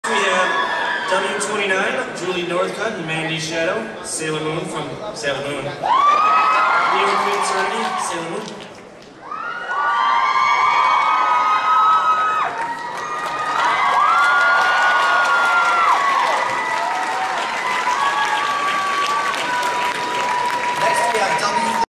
Anime Central 2010 - #29 SM
Video of Entry 18, cosplay walk on of Sailor Moon performed at Anime Central 2010 .  (Permission granted)